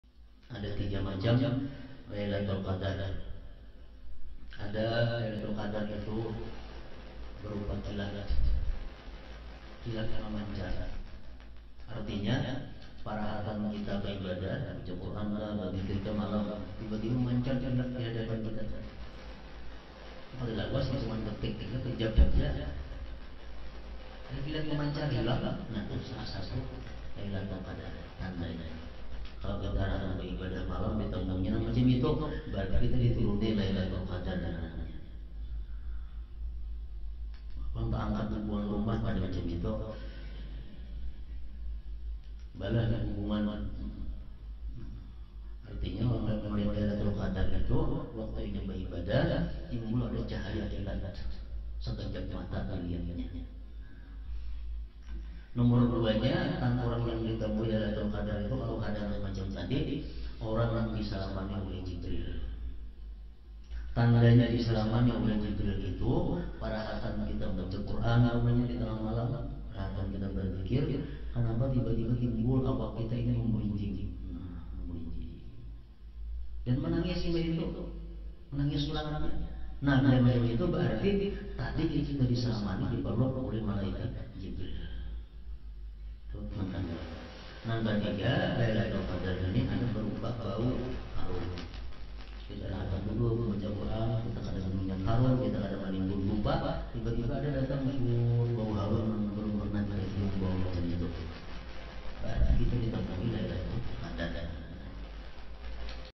Ada 3 Macam Lailtul Qadar || K H Muhammad Bakheit
Kajian TareKat Hakekat Ma'rifat Tasauf
Jalan Mengenal Siapa Allah Siapa Rasulullah
Agar mendapat  iklas yang sempurna
kunci hidayah adalah tauabat